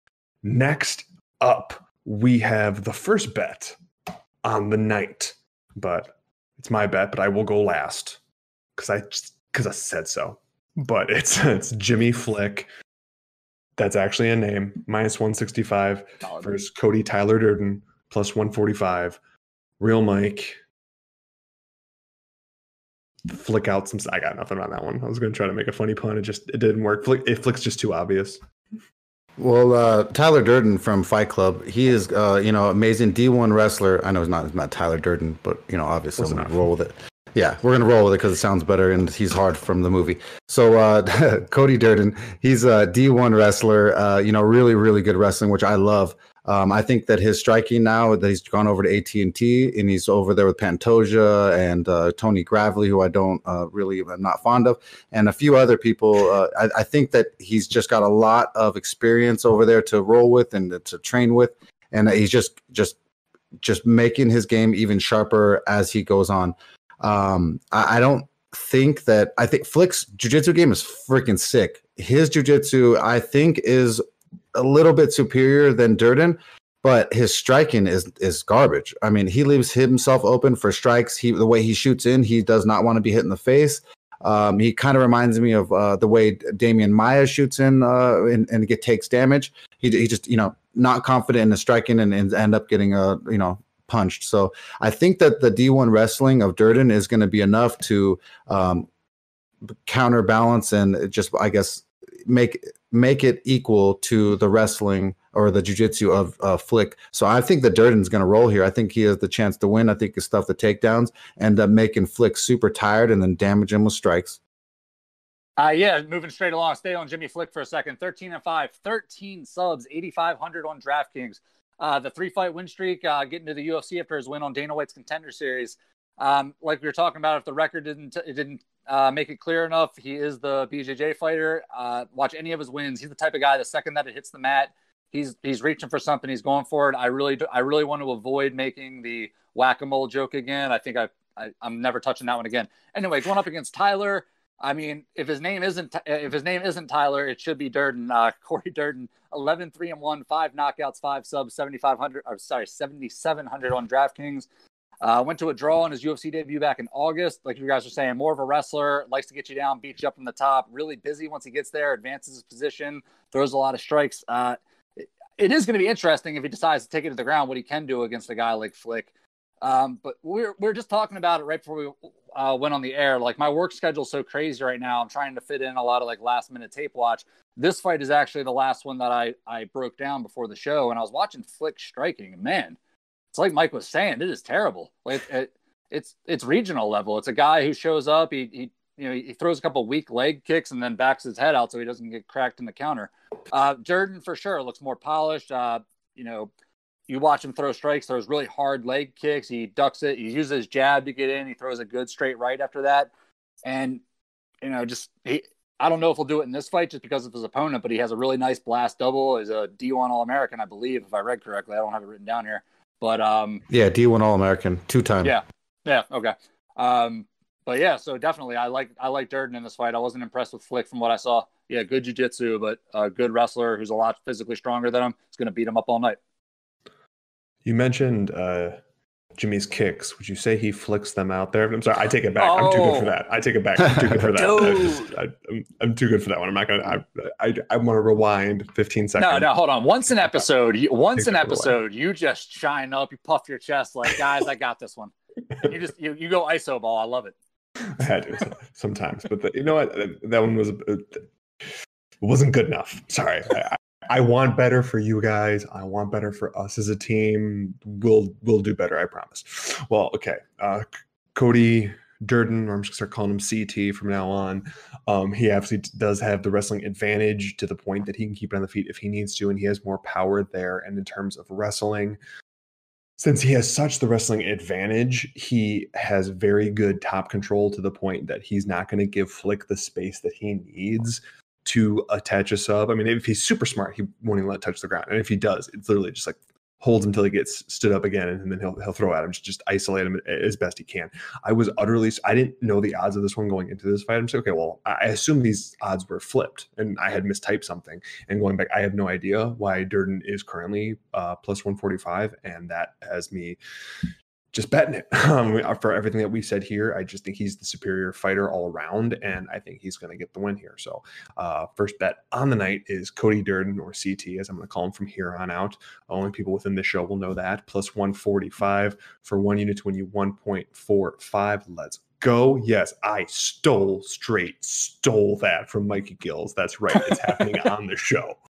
UFC Vegas 16 | Jimmy Flick Vs Cody Durden Preview & Betting Selection
MMAforMoney's own Bob, Mike, & Mikey preview UFCVegas16 match Jimmy Flick Vs Cody Durden and give out their best betting picks!